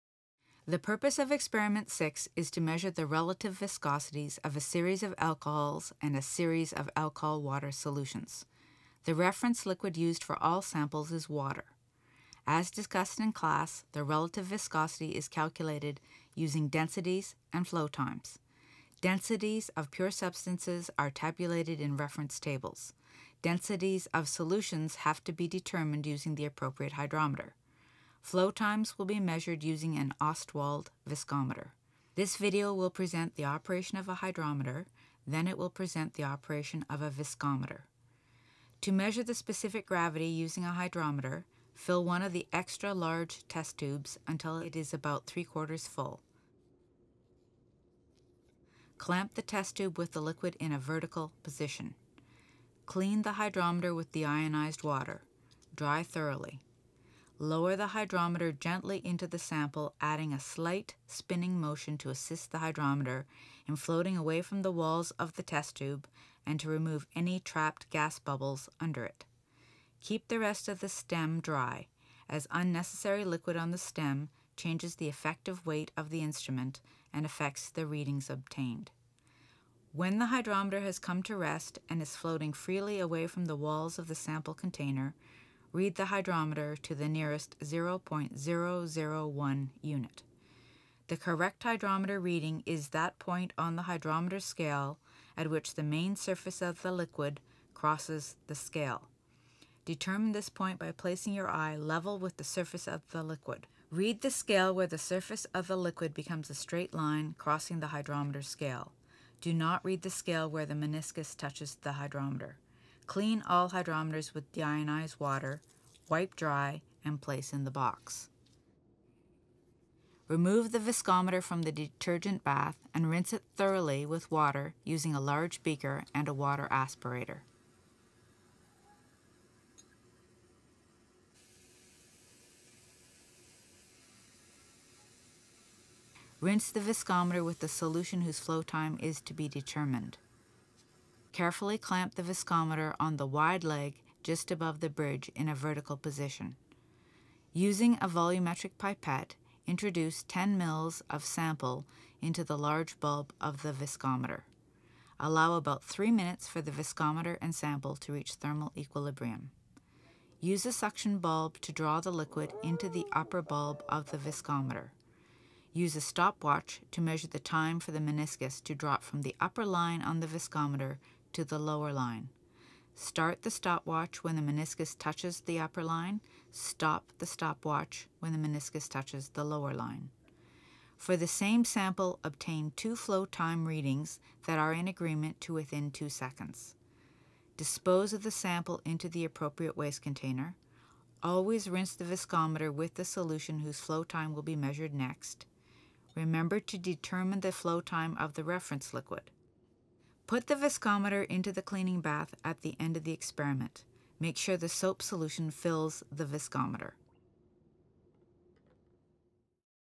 Viscosity
Hydrometer and Ostwald Viscometer Use Instructional Video